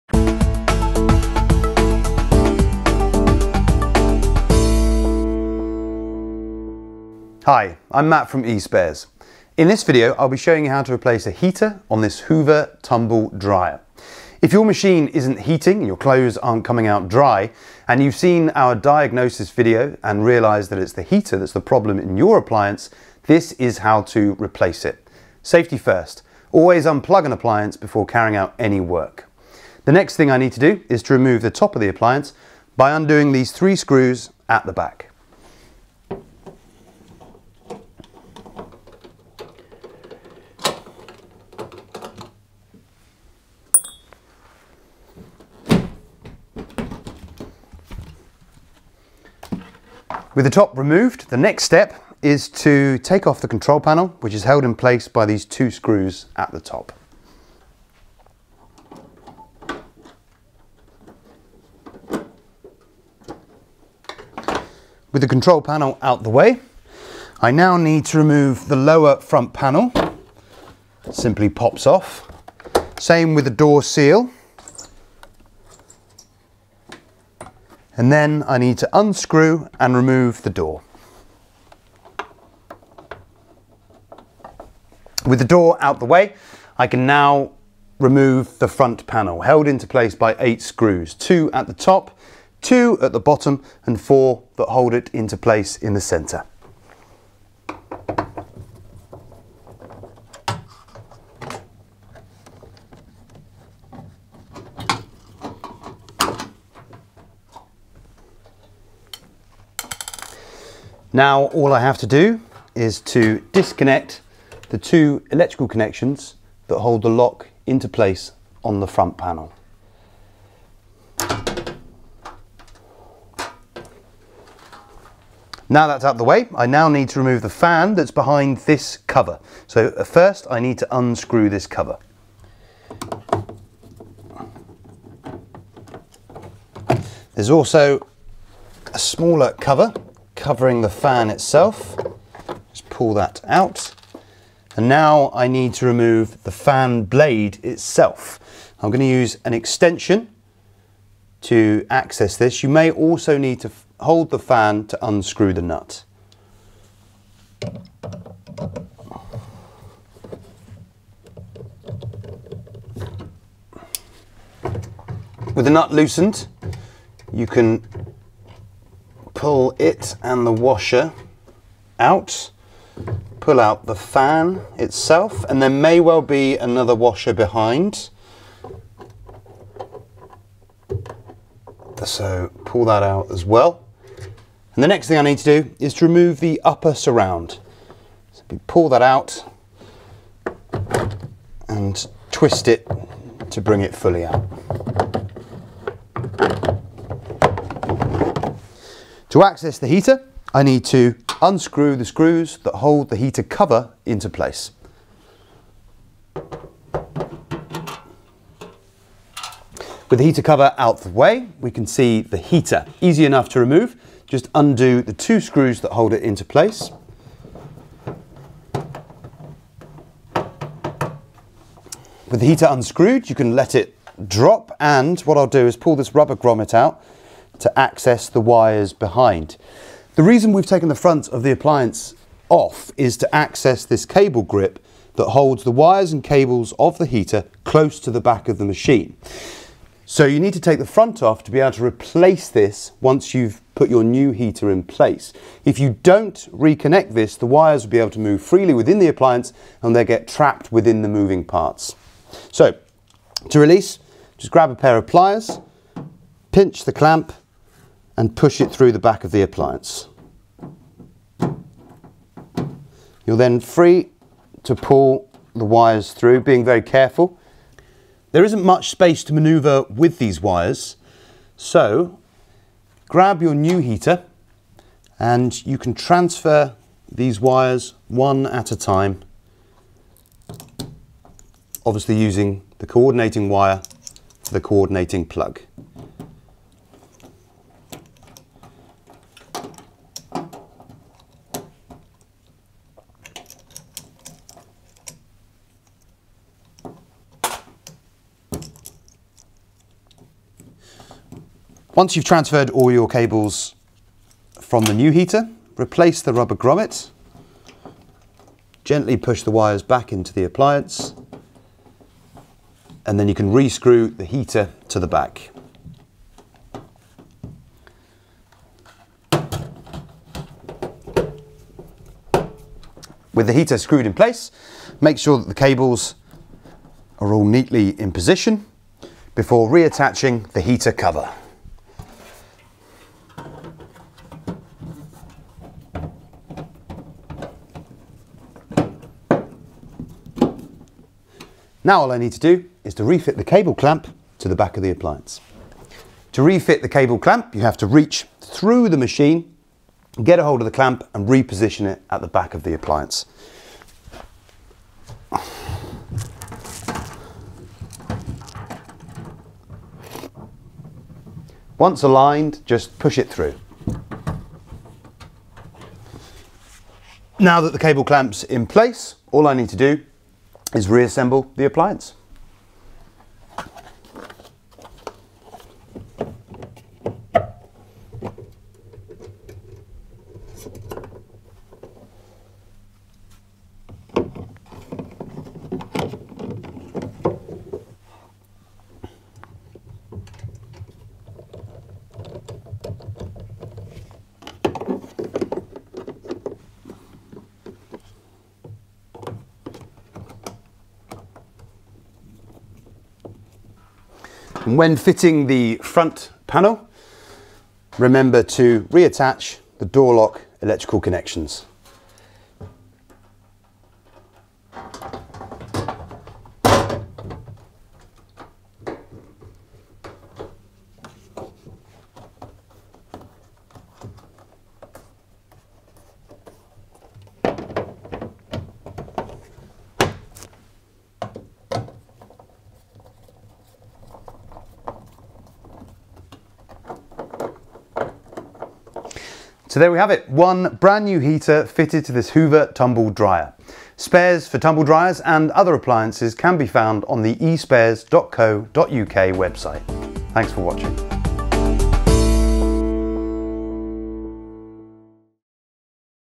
How to Replace a Tumble Dryer Heater (Hoover)
To replace a Hoover dryer heater it may require removing the front to refit the cable clip or the wires may get damaged by the moving parts.
Mat shows how this can be done.

0:00 Introducing the video
0:14 Why the heater may need replacing
0:25 Suggested video: How to check a faulty dryer element using a multimeter
0:29 How to remove the top panel and control panel
1:05 How to remove the lower front panel and door seal
1:18 How to remove the door
1:25 How to remove the front panel
2:05 How to remove the fan covers and fan
3:19 How to access the heater
3:30 How to remove the old heater
4:32 How to fit the new heater
5:23 Putting the tumble dryer back together